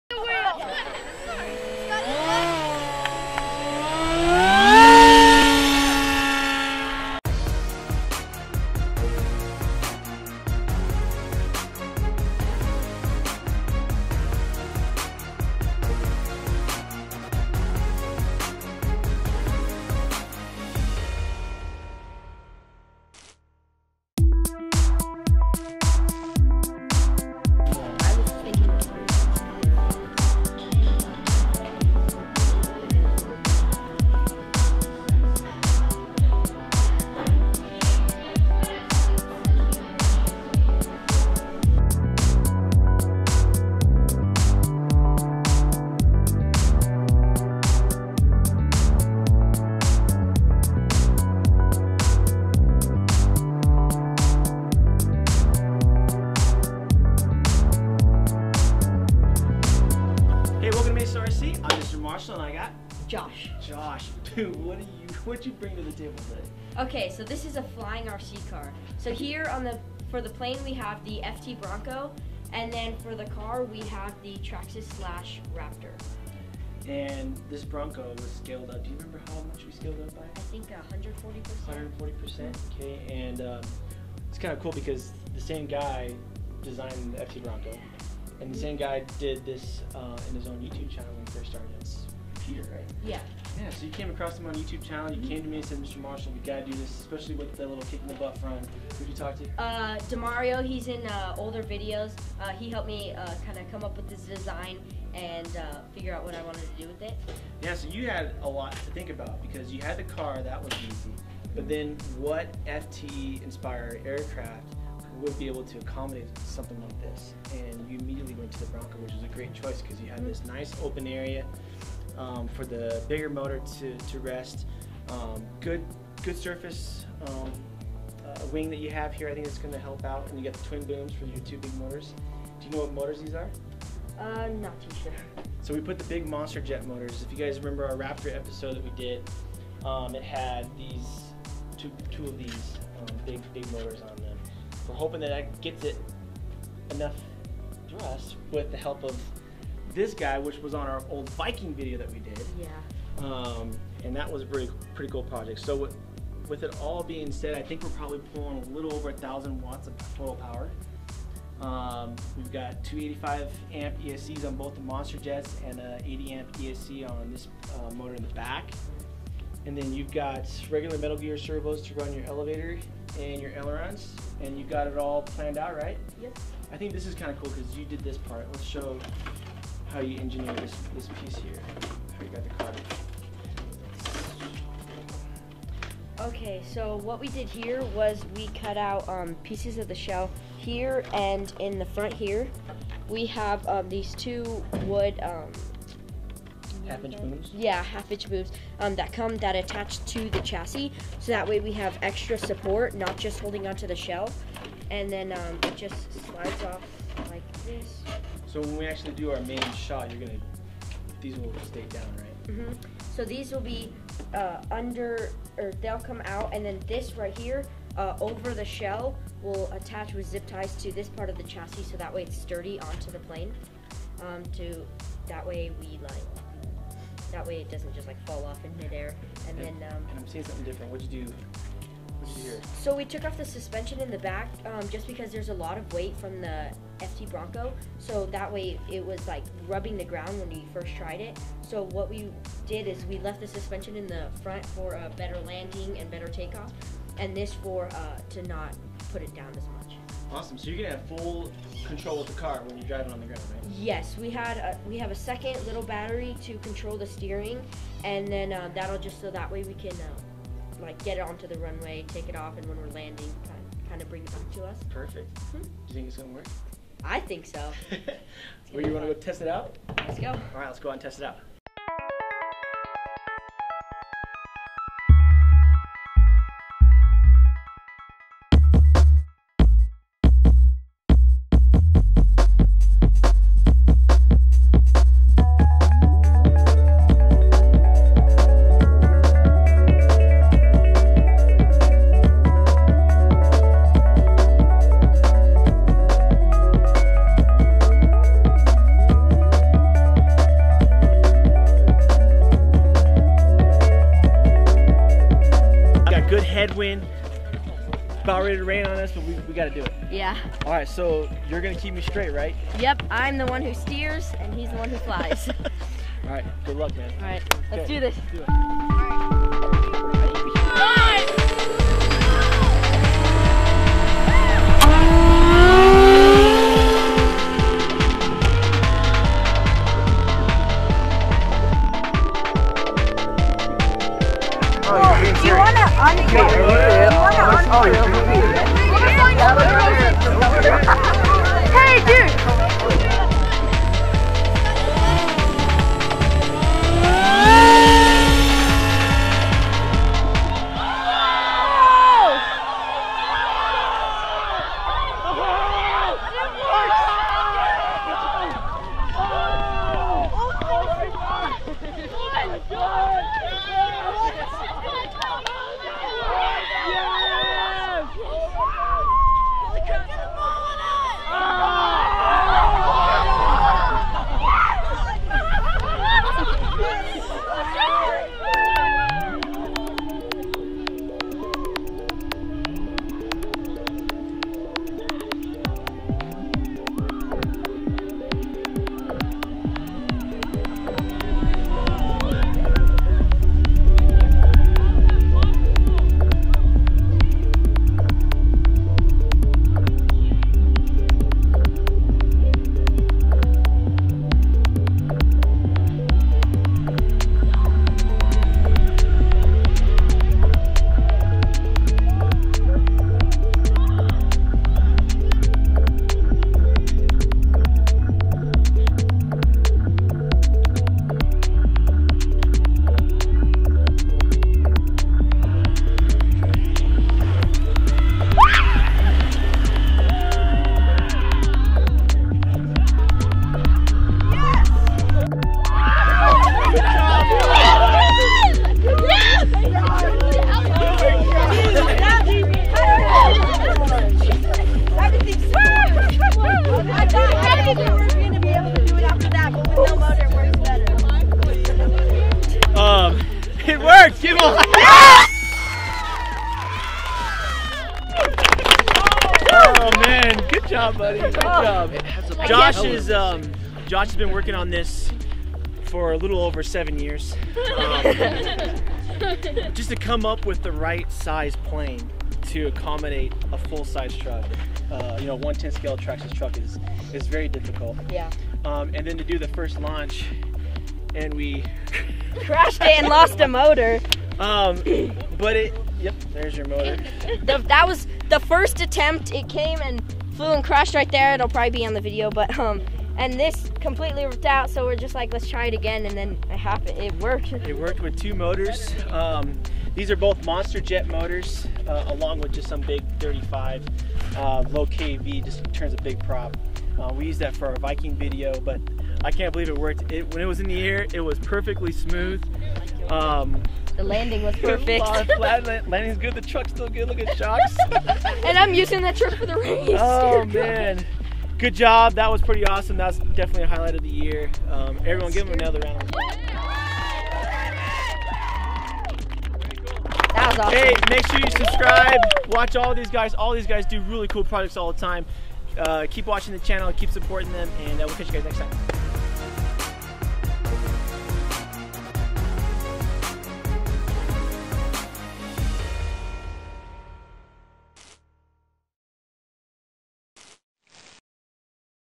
Ultimate RC Flying truck!
Episode featuring MESArc student Josh and his flying truck project.